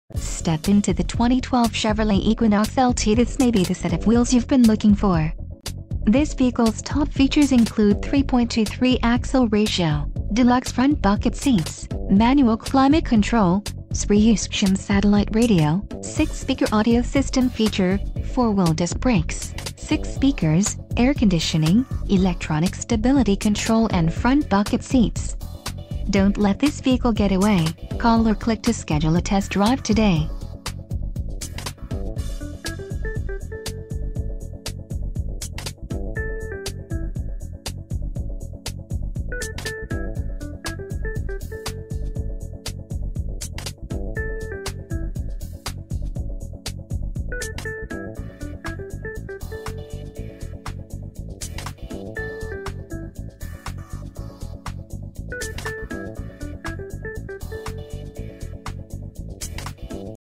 2012 Chevrolet Equinox LT - Veterans Ford - Tampa, FL 33625
Driver Convenience Package (8-Way Power Driver Seat Adjuster and Remote Vehicle Starter System) and Pioneer Premium 8-Speaker System. Yes! Yes! Yes! Clean AutoCheck (No Accident data) Want to stretch your purchasing power? Well take a look at this stunning 2012 Chevrolet Equinox. Having had only one previous owner means that this superb Equinox is sure to be a favorite among our more educated buyers. This SUV is nicely equipped with features such as Driver Convenience Package (8-Way Power Driver Seat Adjuster and Remote Vehicle Starter System) and Pioneer Premium 8-Speaker System. Motor Trend Buyer's Guide reports Equinox's long wheelbase and standard V-6 engine make it one of the more interesting vehicles in its segment. Nobody beats a Veterans Ford deal!!